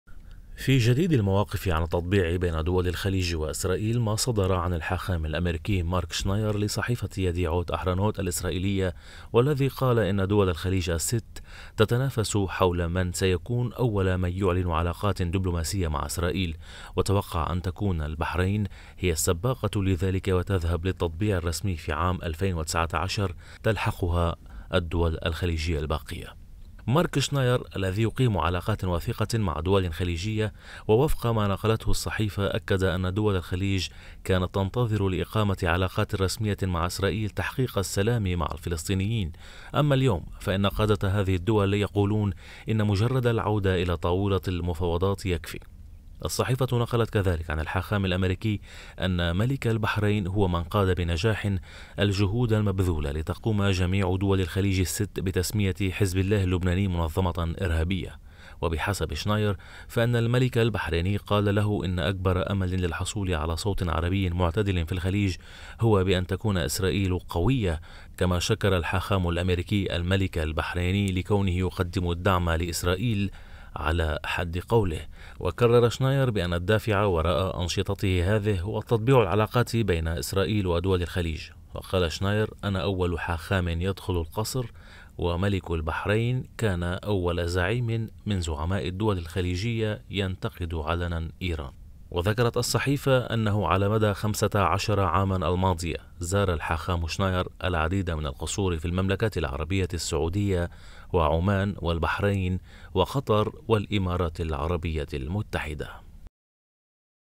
حاخام يتوقع أن تطبع البحرين رسمياً وعلنياً مع "اسرائيل" مطلع العام المقبل
في جديد المواقف عن التطبيع بين دول الخليج و"اسرائيل"، ما صدر عن الحاخام الامريكي مارك شناير لصحيفة يديعوت احرونوت العبرية والذي قال ان دول الخليج الست تتنافس حول من سيكون اول من يعلن علاقات دبلوماسية مع اسرائيل.

وتوقع شناير في حديثه للصحفية ان تكون البحرين هي السباقة لذلك وتذهب للتطبيع الرسمي في عام 2019 وتلحقها الدول الخليجية الباقية.